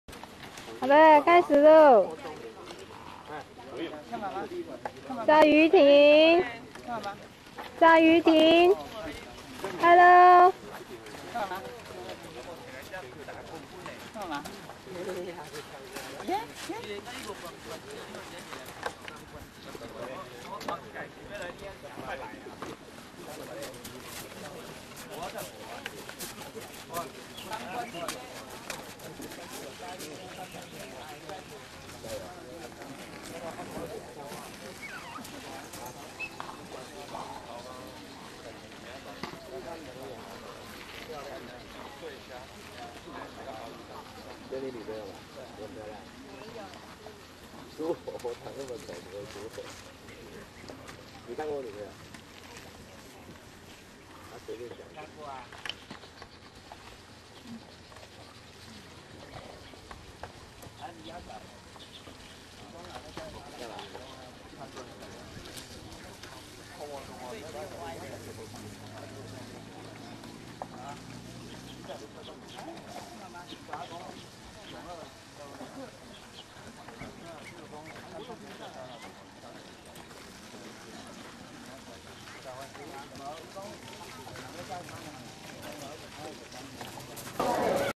是女兒不是兒子
87年區運會馬術  騎士趙其煊 馬名雷射
台南 新化 虎頭碑馬場